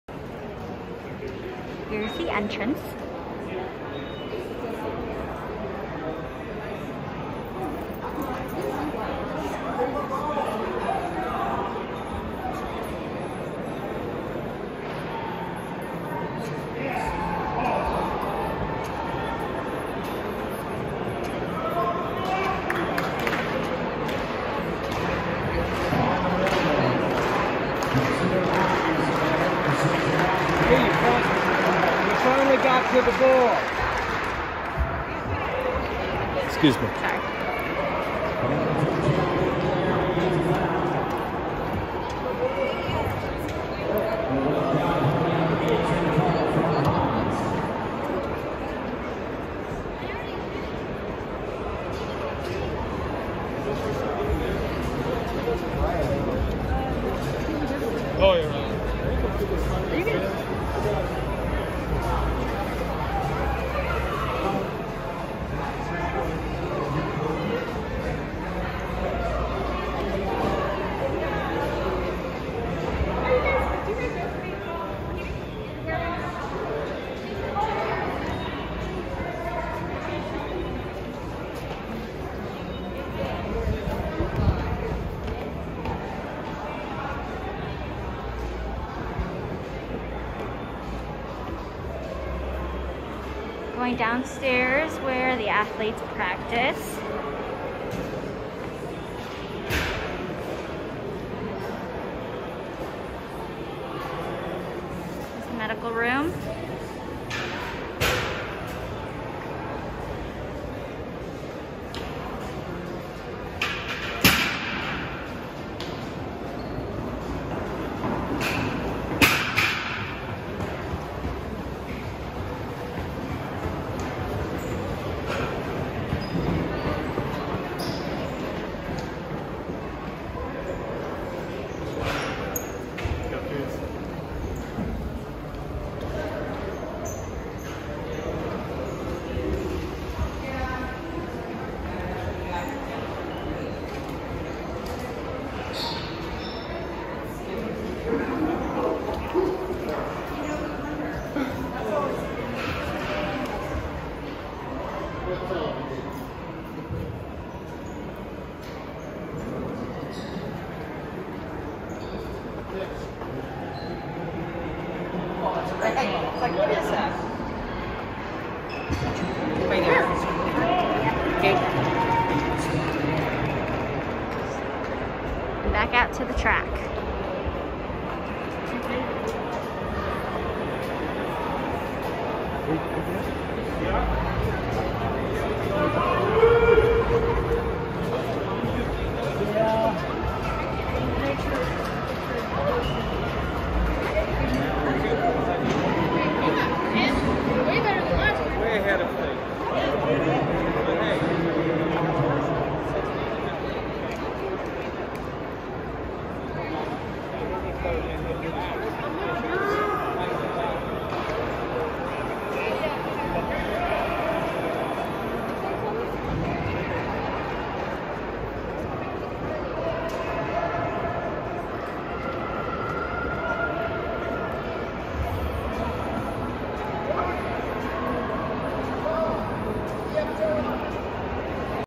RAW: Take a tour of The Podium in Spokane | KXLY
The Podium in Spokane, Washington, is finally open. The Podium held its first event on Dec. 11, 2021. The Podium is a unique competition venue designed for ultimate athlete and fan convenience.

Take a look inside with 4 News Now KXLY reporter Elenee Dao.